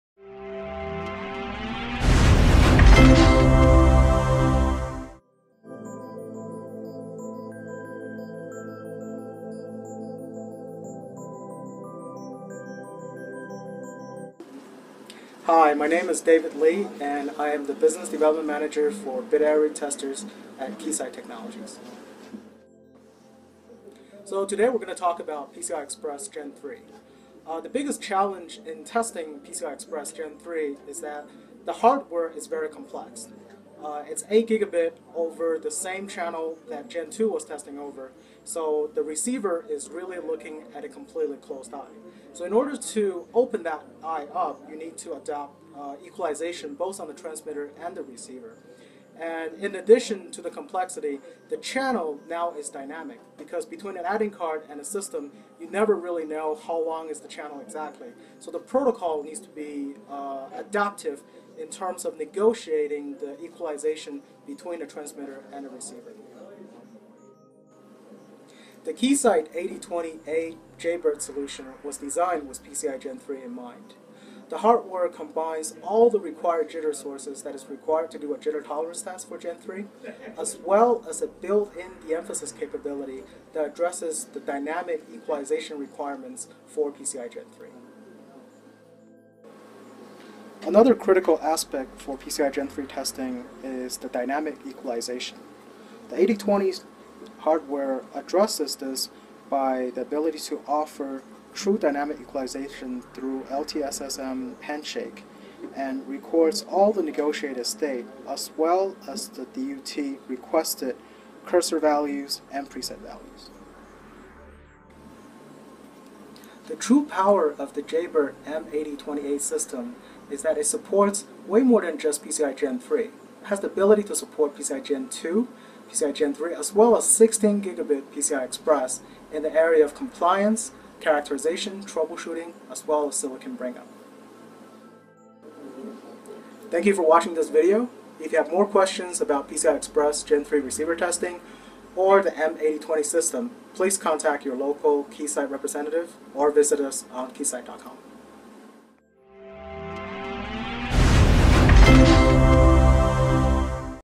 Keysight Technologies PCIe Receiver Compliance Test Solutions
This video explains PCIe Gen 3 and the challenges associated with receiver compliance. Receivers are looking at a completely closed eye , to open the eye up, users need to adopt equalization on both the transmitter and receiver.